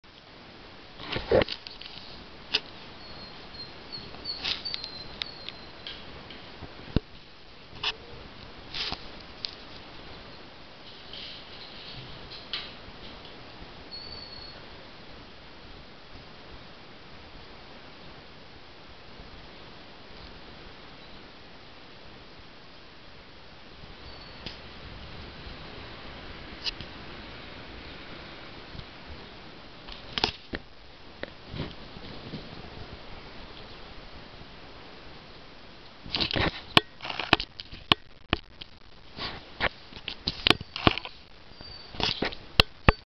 Pisa nas alturas - Pisa in the heights
Quando se é uma bisbilhoteira nata e não nos deixam ir para a rua, há que encontrar outras formas de ver o que se passa lá fora... A nossa cadela, a Pisa, foi capaz de resolver a situação!